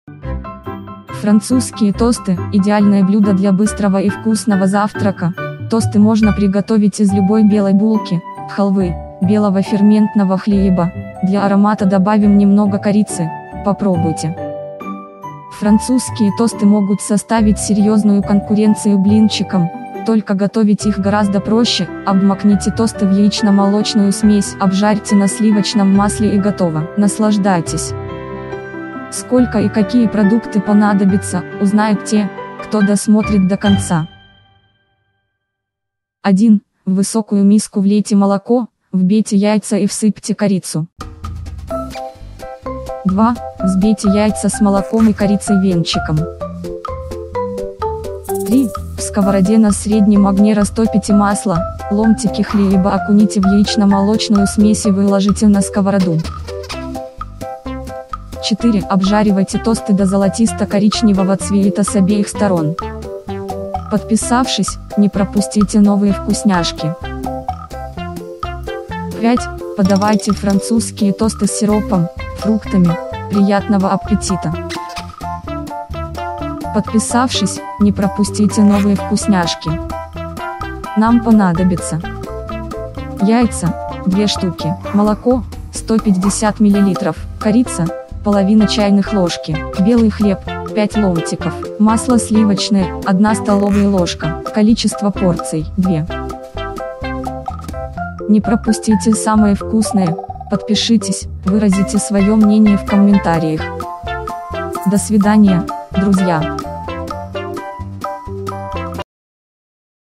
Французские тосты Рецепт, пошаговый, как приготовить, видеорецепт
Лучшее - для Вашей кухни. Французские тосты - идеальное блюдо для быстрого и вкусного завтрака. Тосты можно приготовить из любой белой булки, халвы, белого ферментного хлеба. Для аромата добавим немного корицы. Попробуйте!